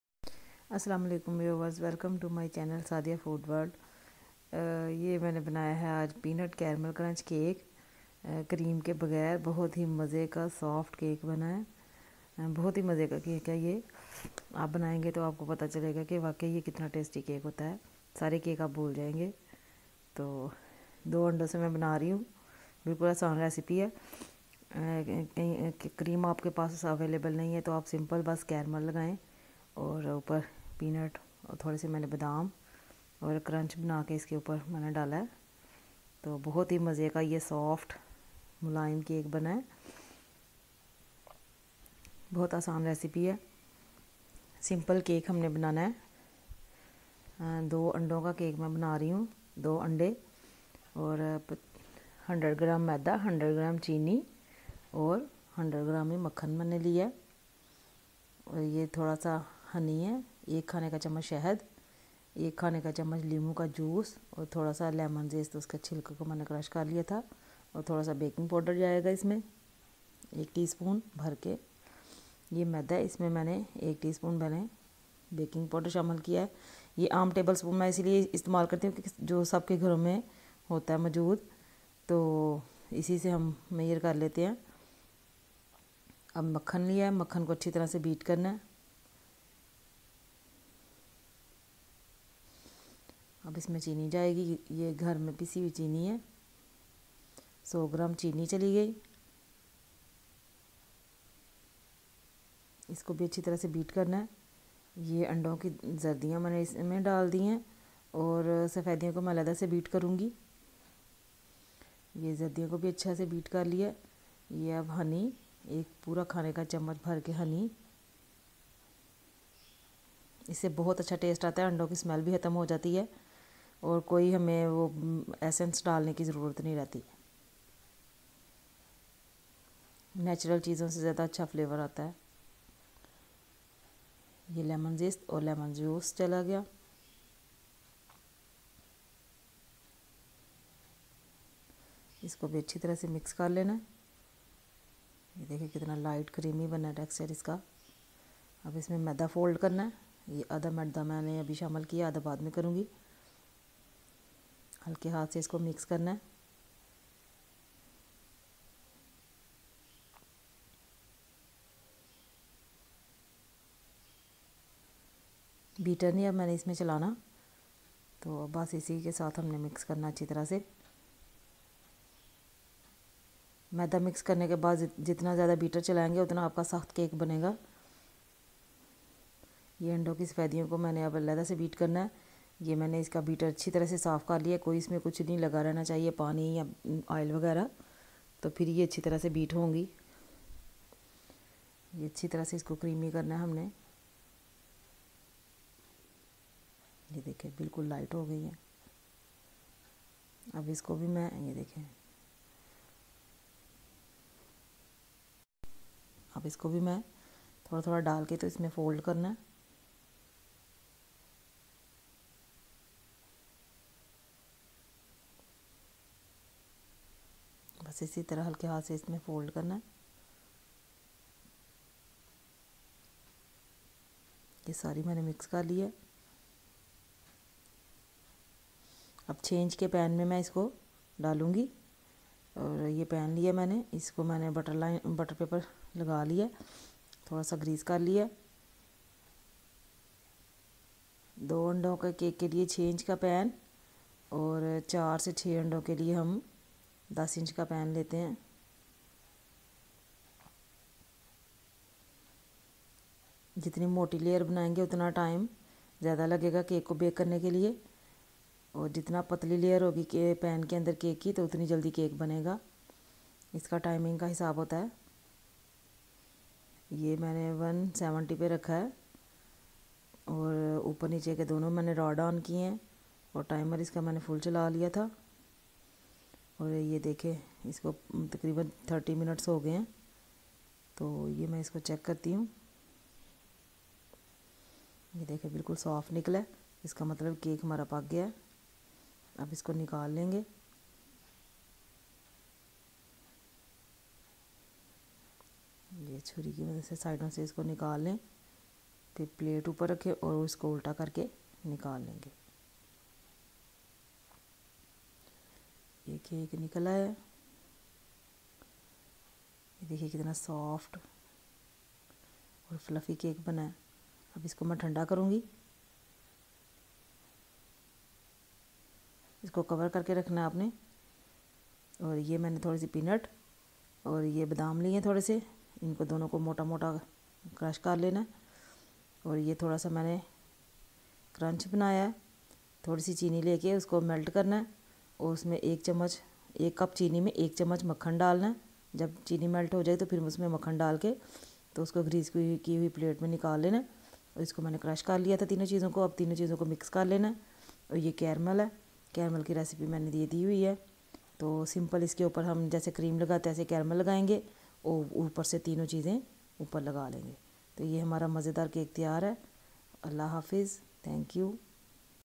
Peanut caramel crunch cake (with out cream)
Hello friends in this video we are going to make a very special recipie

Peanut carramel crunch cake

It is a very delicious cake and hope that all your familly members would love it
Enjoy  also if you have any questions or want any recipie just give us a comment